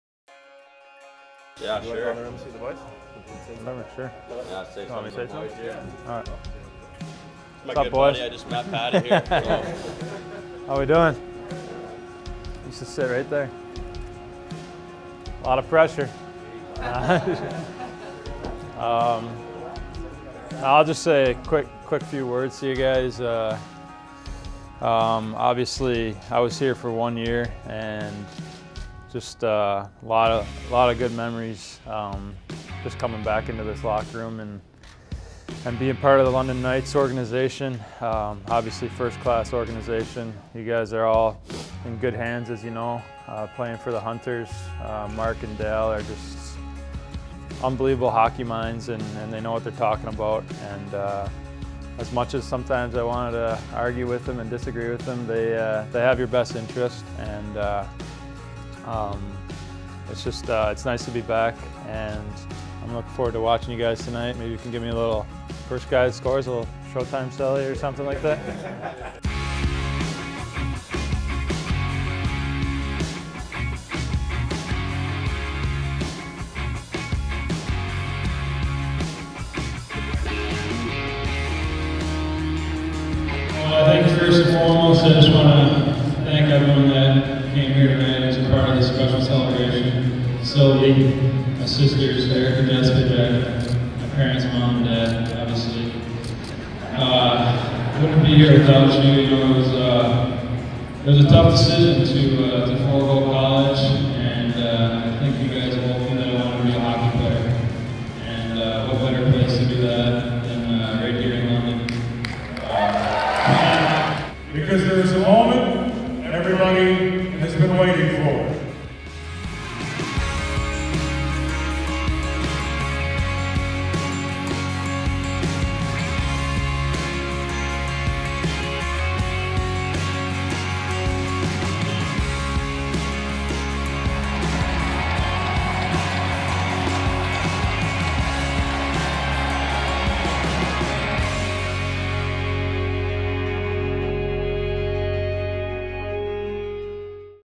Behind the Scenes: Patrick Kane Jersey Retirement
On January 17th, 2020, the London Knights retired the number 88 in honour of the career and accomplishments of Patrick Kane.

Watch as Patrick Kane addresses the current London Knights team in the dressing room and then see his number hang from the rafters.